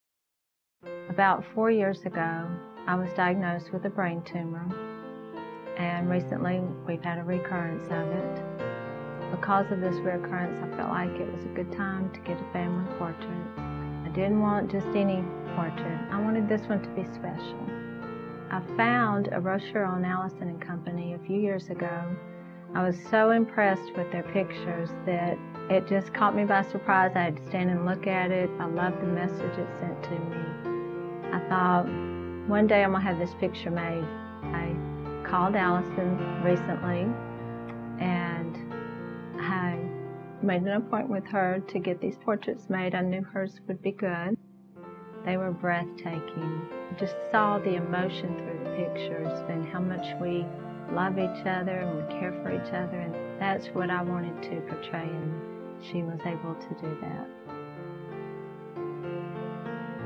Allison and Company
Television Commercial for Allison and Co. Photography Studio.  A high-end photography studio specializing in family portraits.

Producer/Writer/Creative Director: Craig Tilley